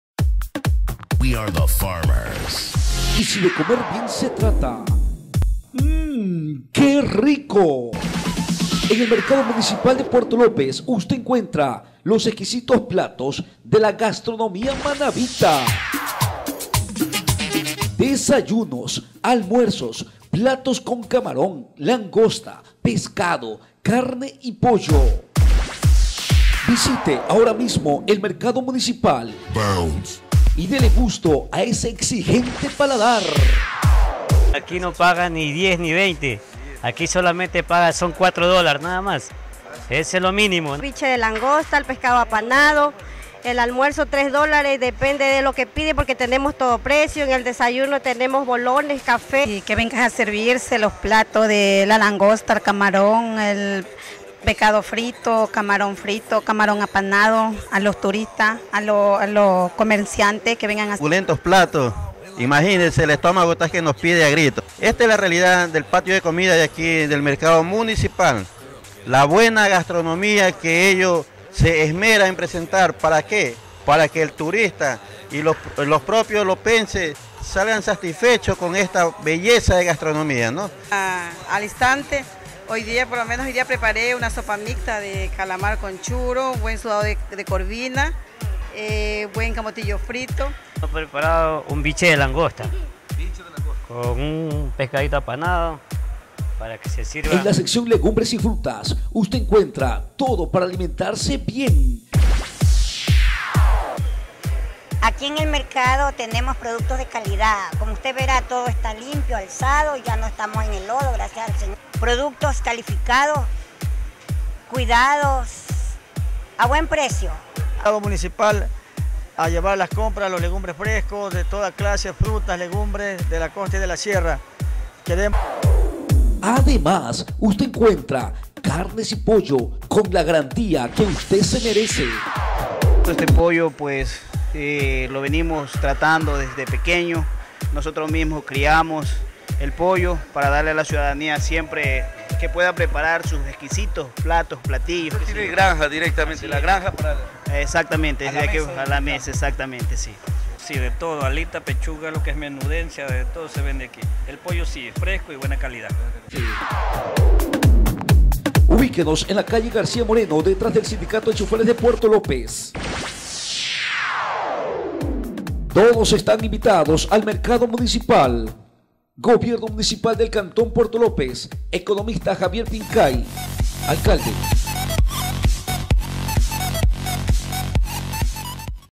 EXQUISITA GASTRONOMÍA SE PREPARA EN EL MERCADO MUNICIPAL
COMERCIANTES DEL MERCADO OFERTAN PRODUCTOS FRESCOS Y ECONÓMICOS